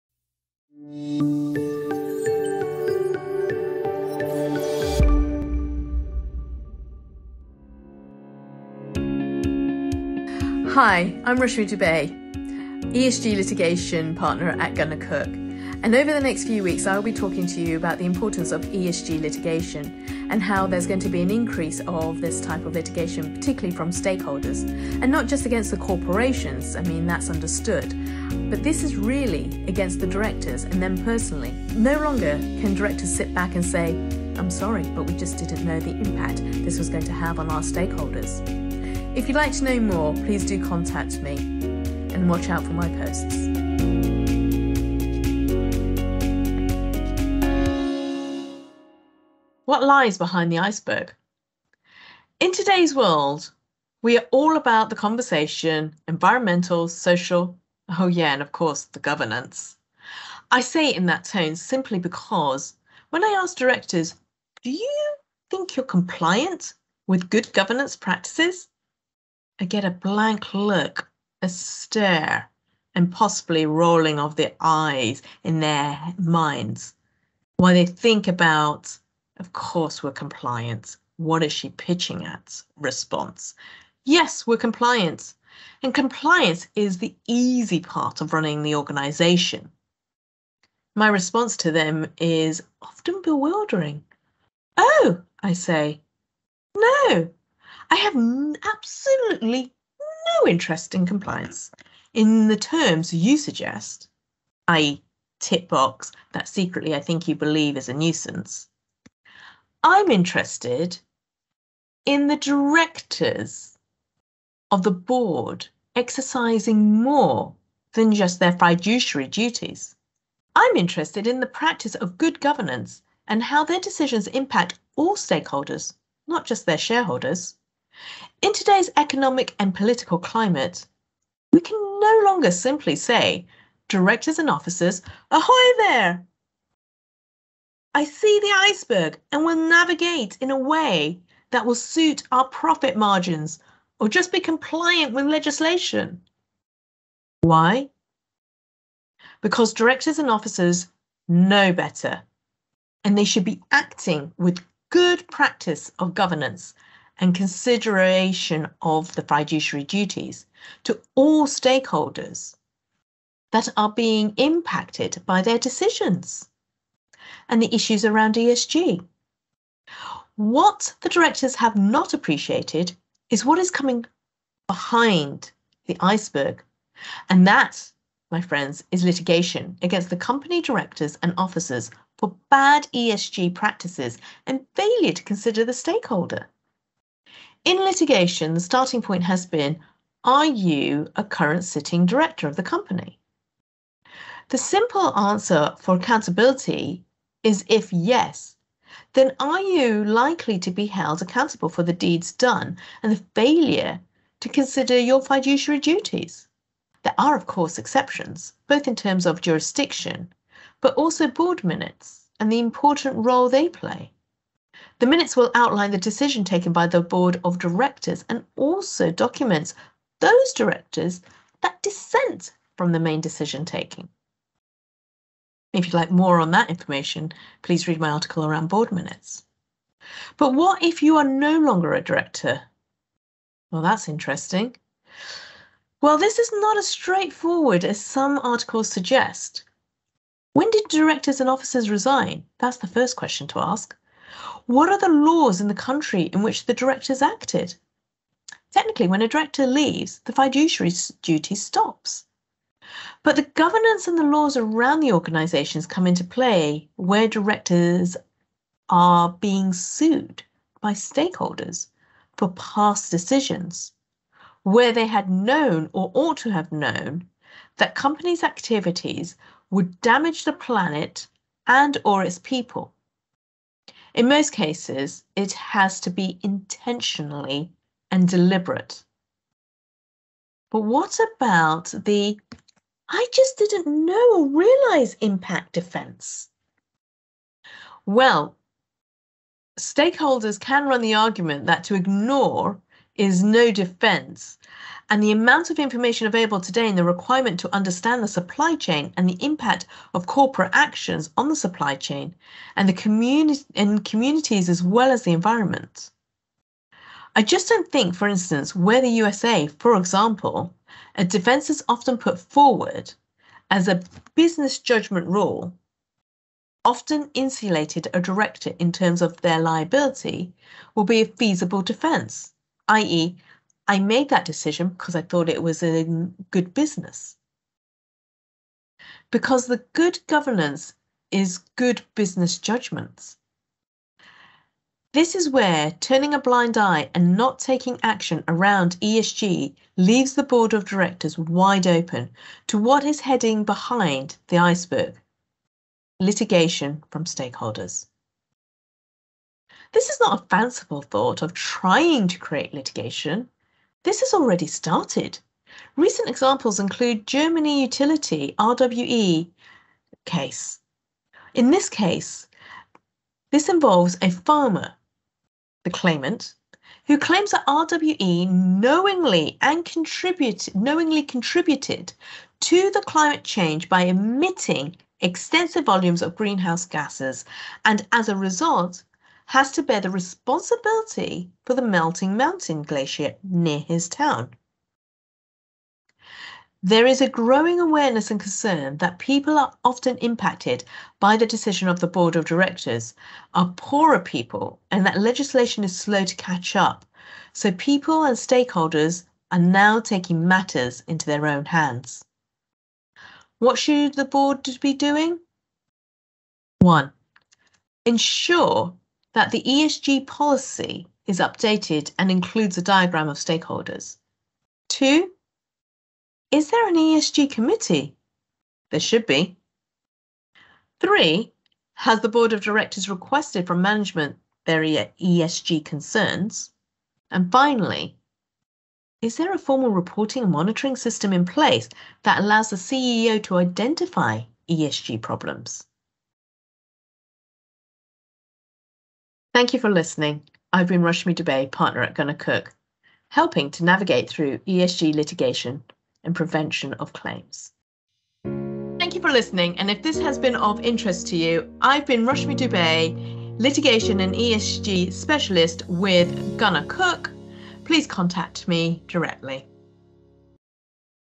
Rashmi Dube - ESG Litigation advice. What lies behind the iceberg?
In the second installment of the series, Rashmi Dube discusses a recent ESG litigation case. You can read the article here